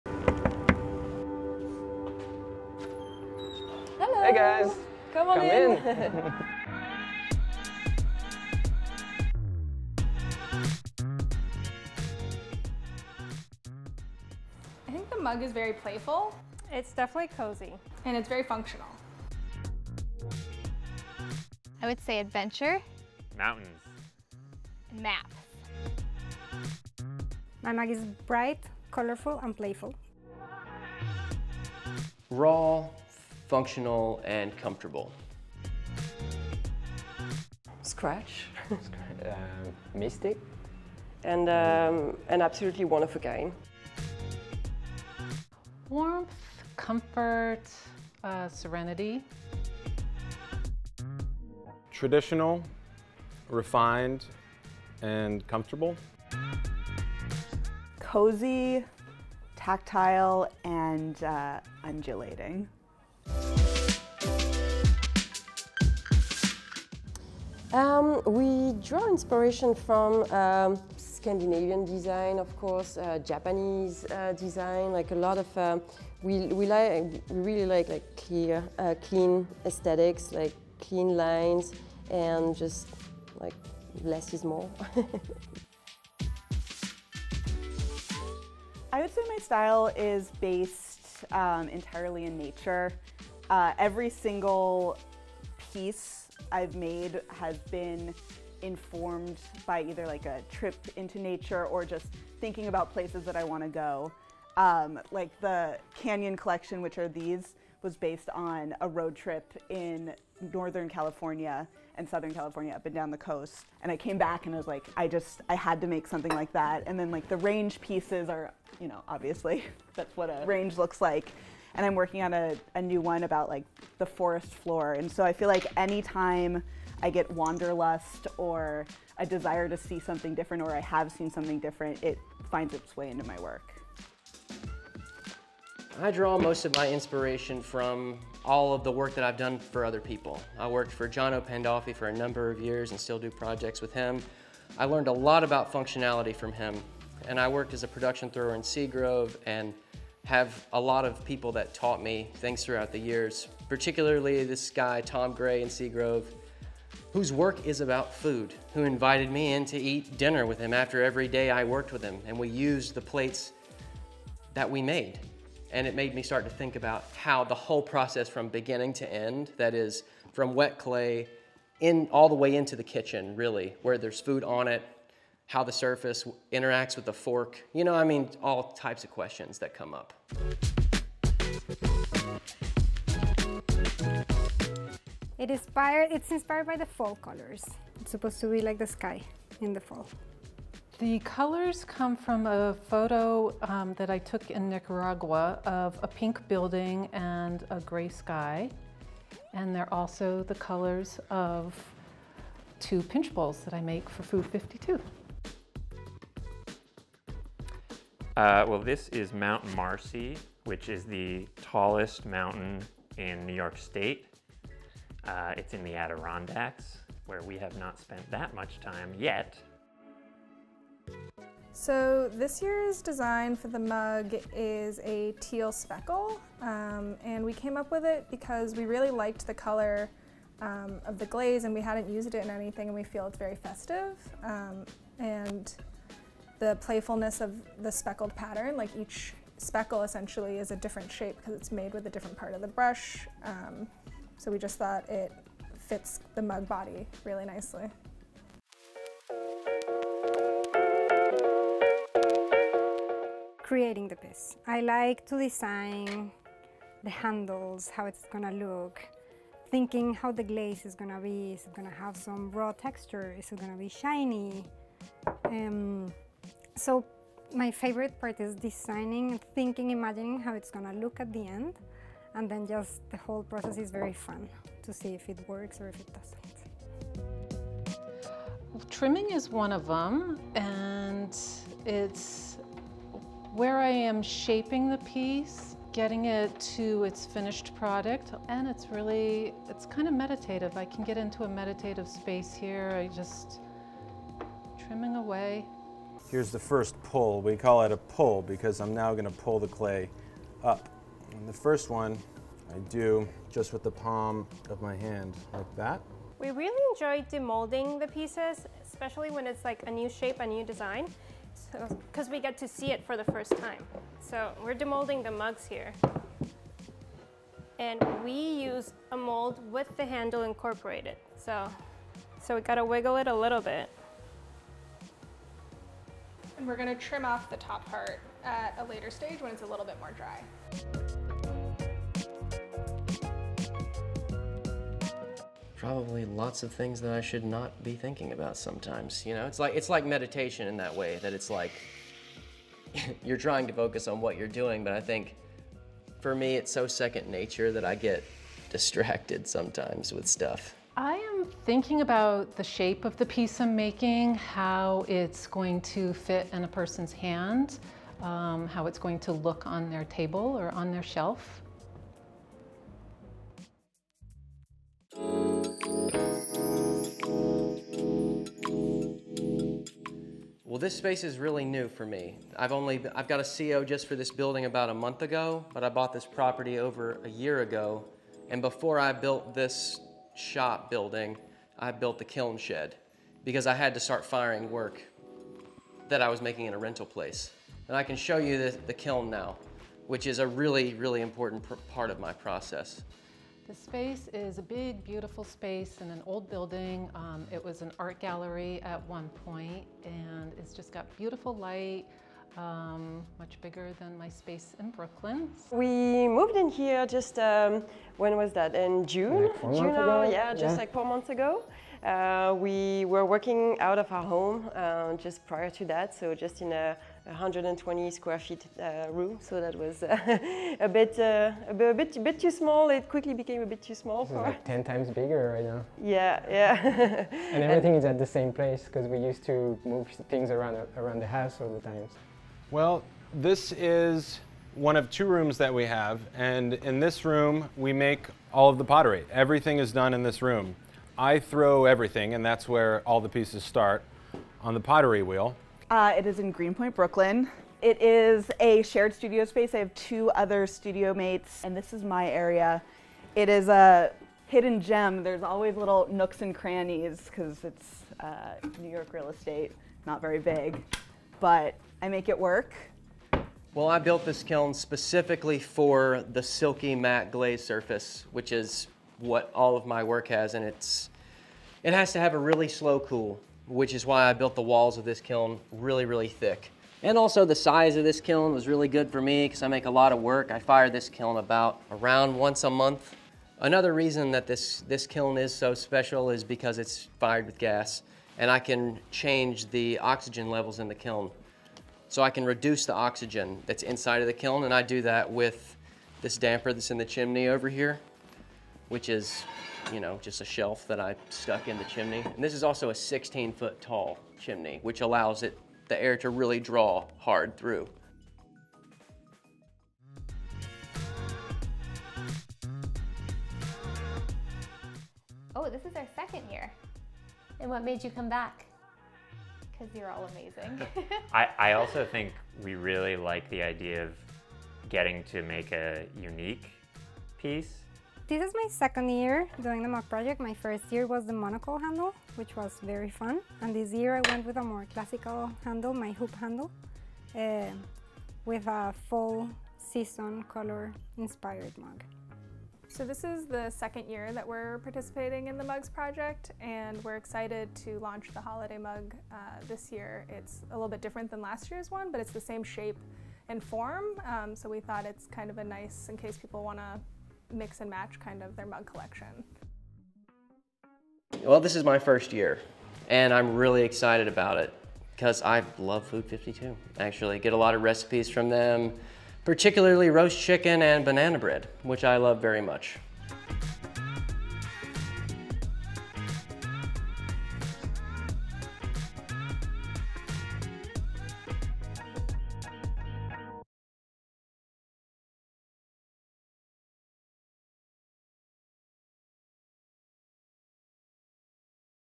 52 Handmade Mugs, 52 Artists, 52 Stories | Mugs52
Welcome to Mugs52, our annual collection of just-for-Food52 cups of art, created by 52 independent makers. We visited some of the ceramicists who handcrafted our mugs, in Bushwick pottery studios and at-home workshops in upstate New York. They’re solo artists, married couples, and lifelong friends, each with their own experiences that they turned into dazzling designs.